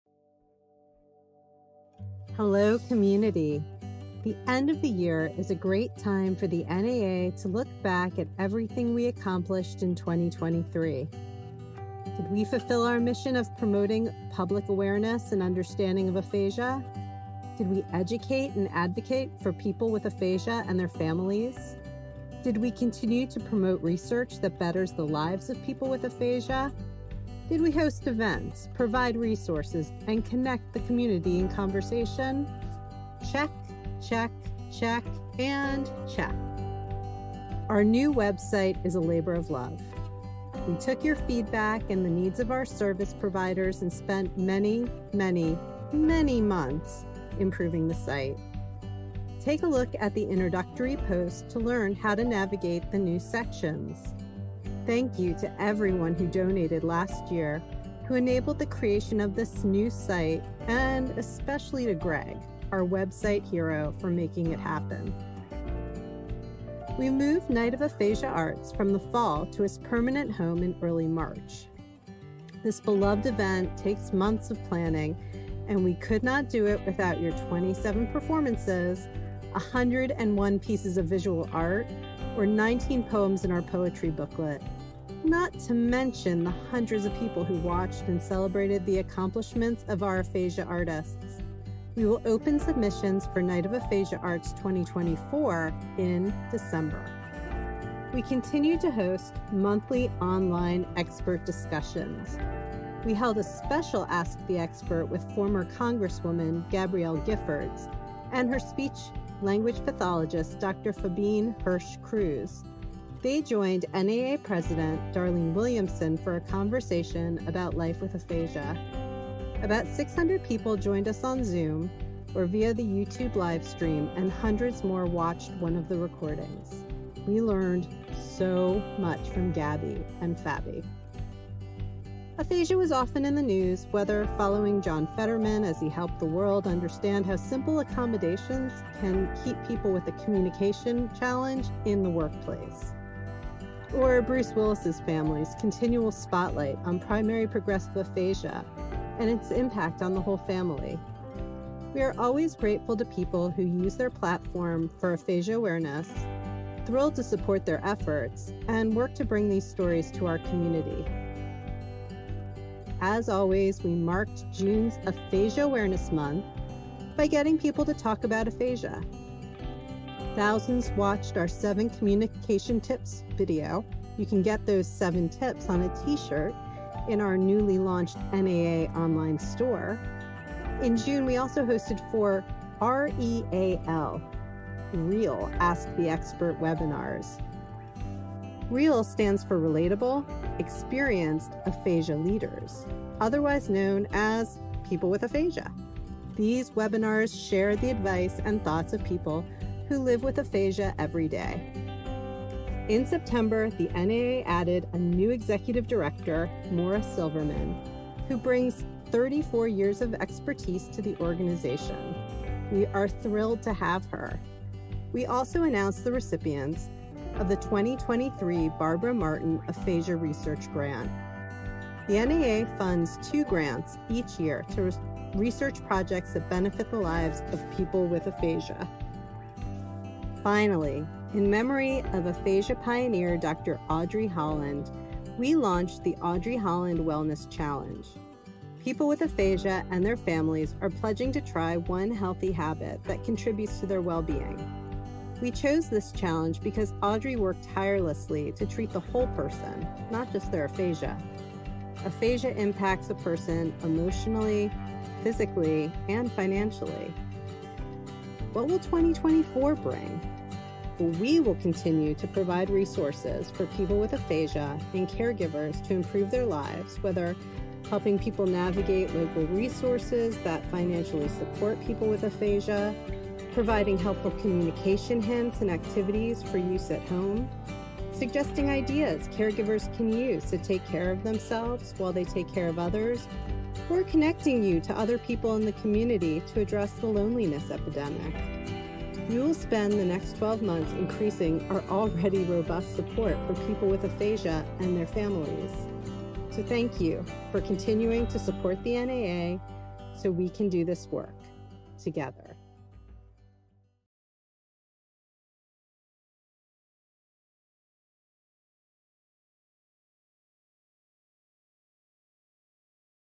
2023 NAA Reflection Video
Hello community! The end of the year is a great time for the NAA to look back at everything we accomplished in 2023. Watch our end of year video.